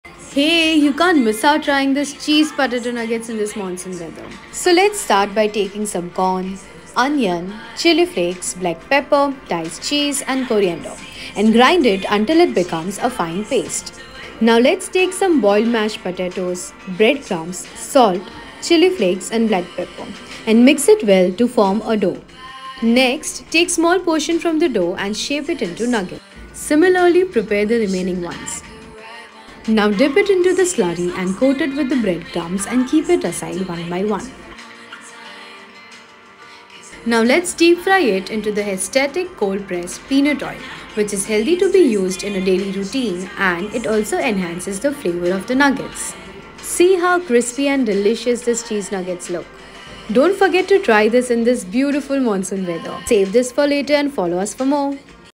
Cheese Potato Nuggets | Party starter recipe | Crispy potato | कुरकुरे आलू नगेट्स घर पर
Cheese Potato Nuggets😍
Try this delicious recipe in this amazing weather with your loved ones!😋⛈

Also, use @hestheticofficial to deep fry the nuggets that will definitely enhance your taste.🤩